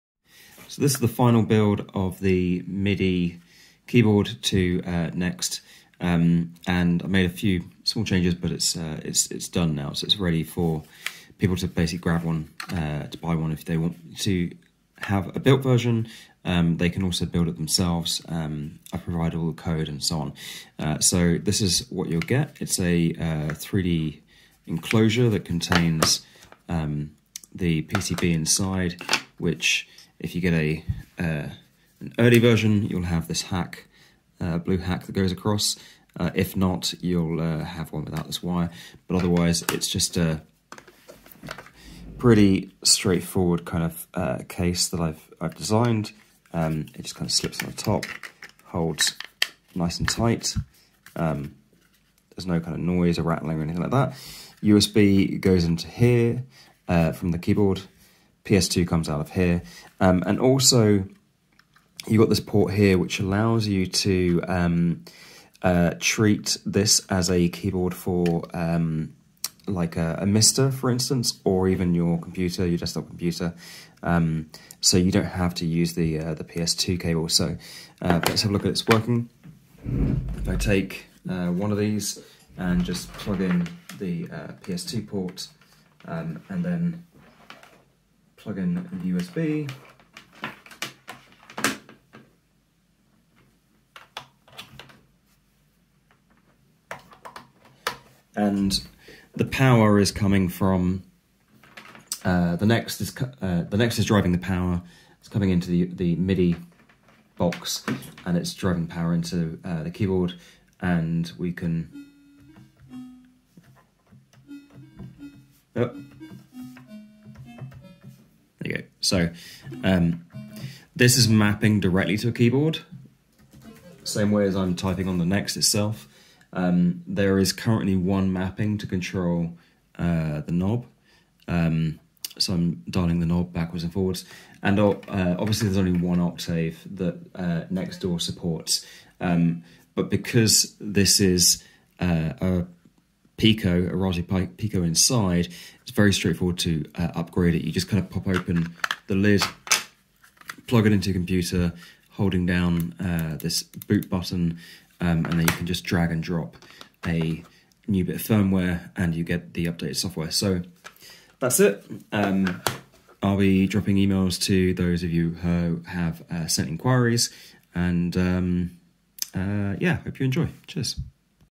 USB midi to ps/2 for Next and NextDAW - final build
MIDI to Spectrum Next. Small mistake in the demo - NextDAW supports two octaves.